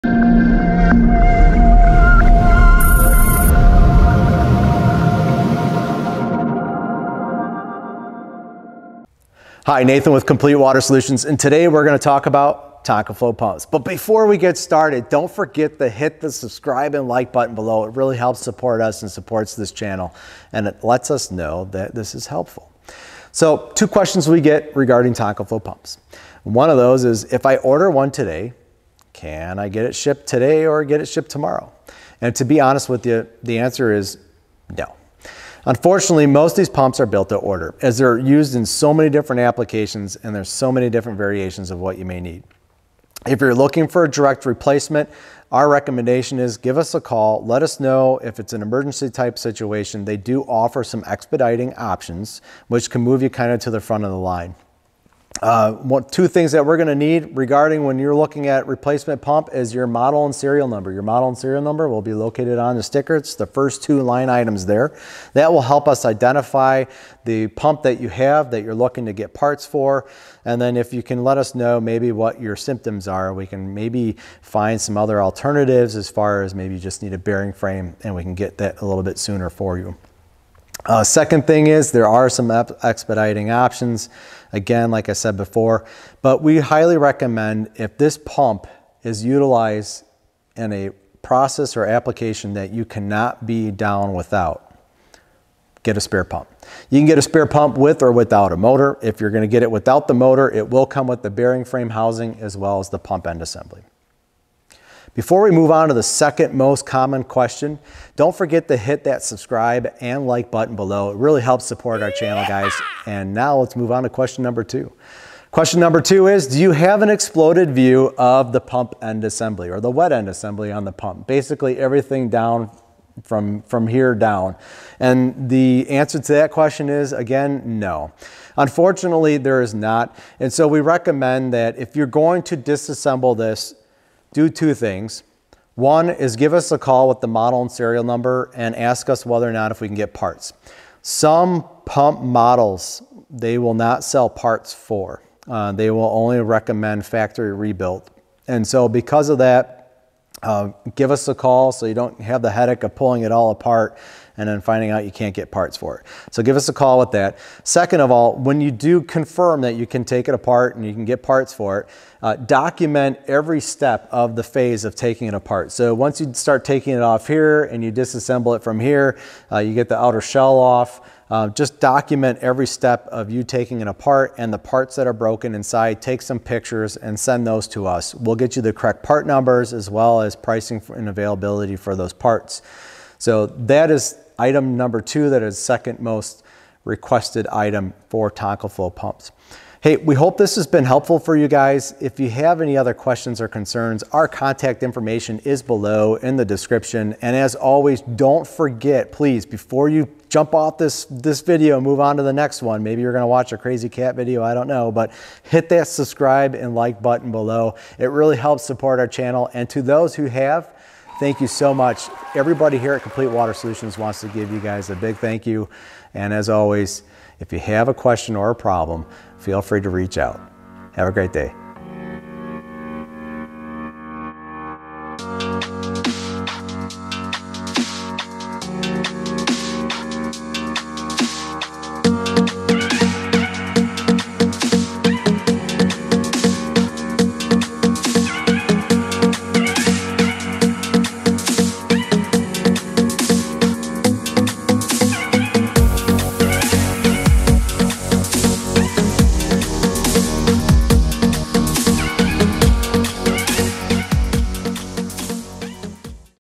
Tonkaflo Pump Drawings & Tonkaflo Pump Manuals - Watch Before Taking Apart
Tonkaflo pumps have been around for over 30+ years and have been the workhorse behind Osmonics RO Systems. In this video we discuss lead times for tonkaflo pumps, spare pumps, and why you want to document taking apart a tonkaflo pump. More importantly why you should call us prior to taking apart your pump.